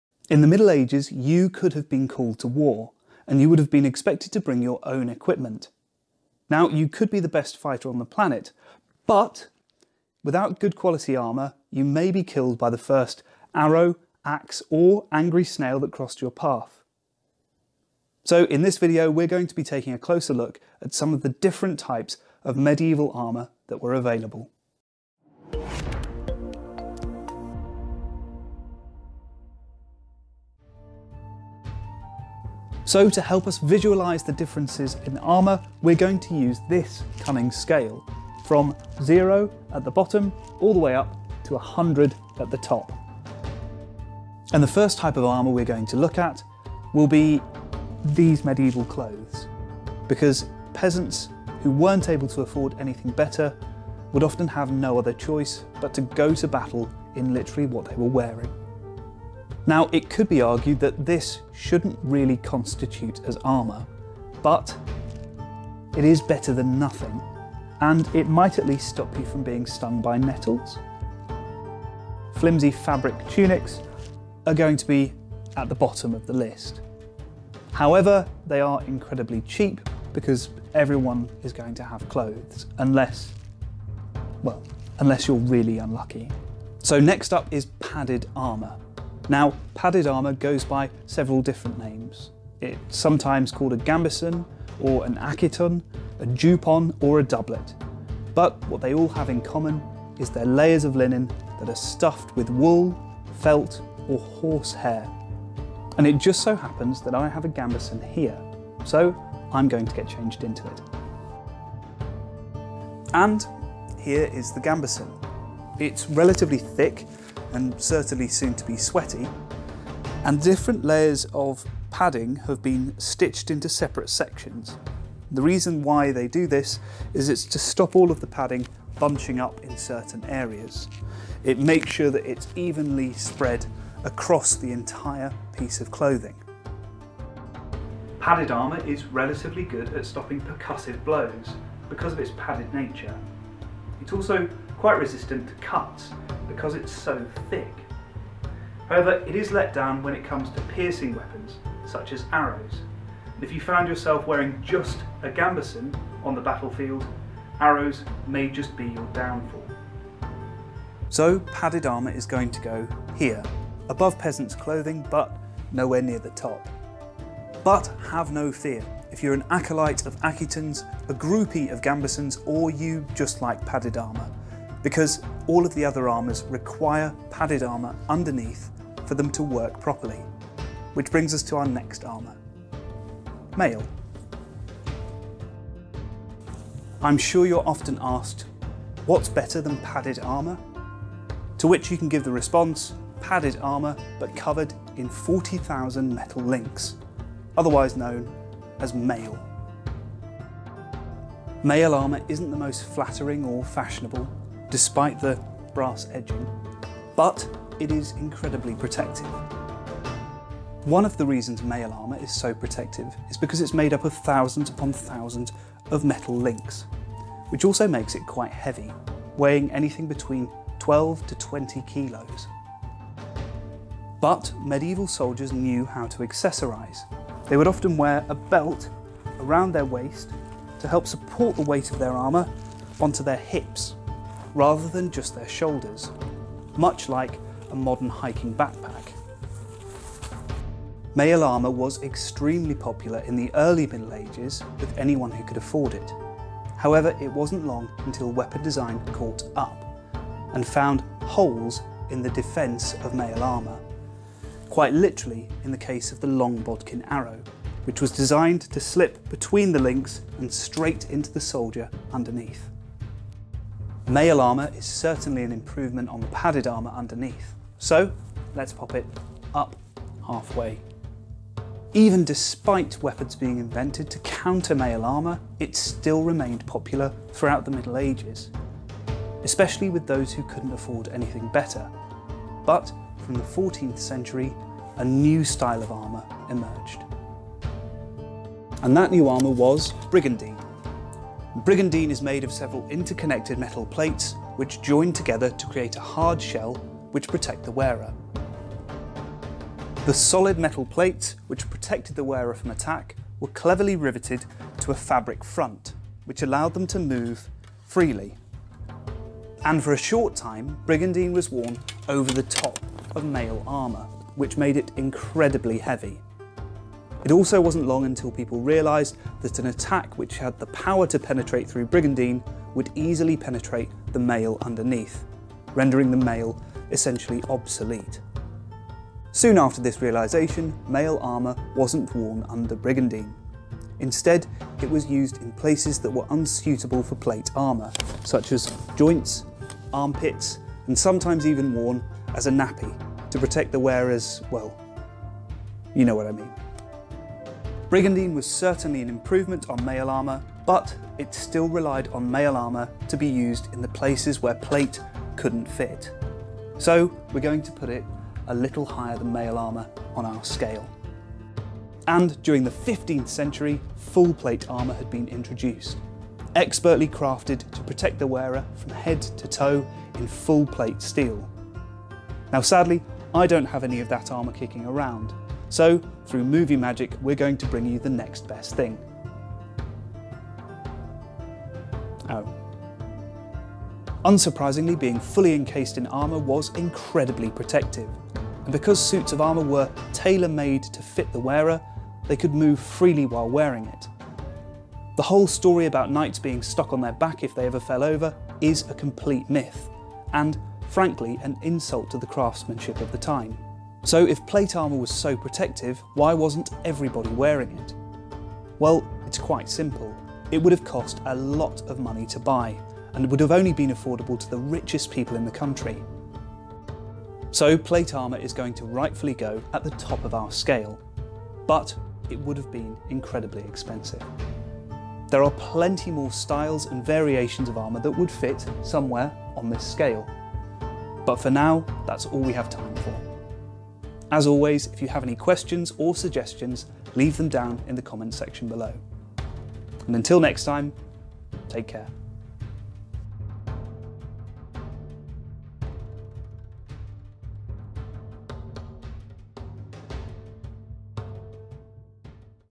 Arms and Armour: Part 2 Medieval Armour
Part 2 of our Arms and Armour series takes a look at different styles of Medieval Armour.